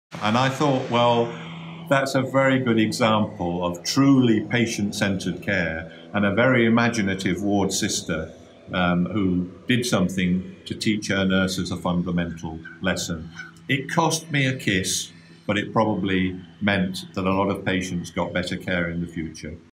How did the NHS cost Professor Sir Liam Donaldson "a kiss"?
Visit Good Governance TV Youtube channel for the full video of Sir Liam Donaldson's interview with GGTV